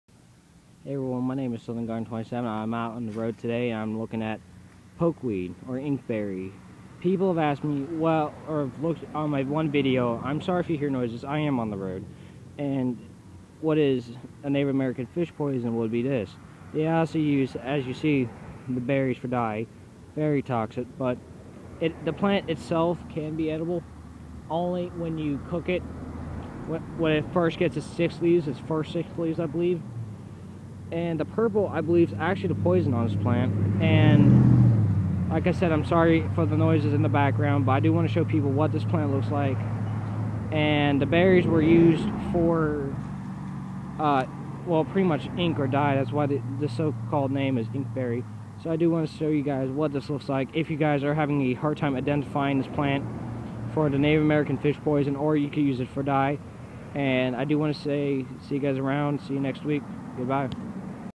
What is a inkberry?
ink berry is toxic is also know as pokeweed and i have another video called organic fish posion